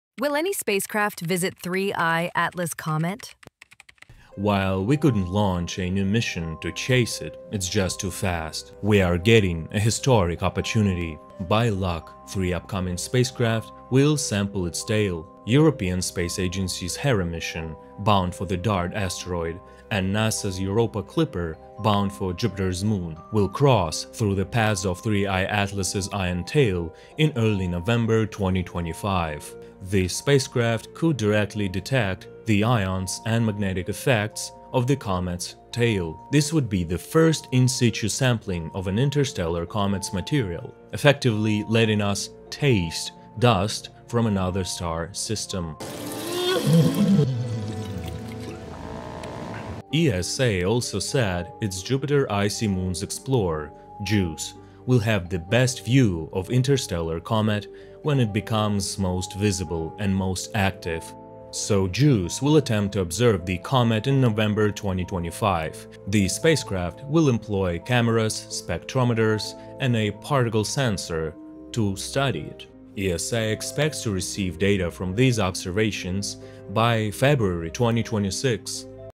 ☄️How We're ACCIDENTALLY Visiting an Alien Comet 3I/ATLAS ☄️| WSO
By pure luck, three spacecraft will actually sample the tail of the interstellar comet 3I/ATLAS, effectively letting us "taste" dust from another star system. What can we learn?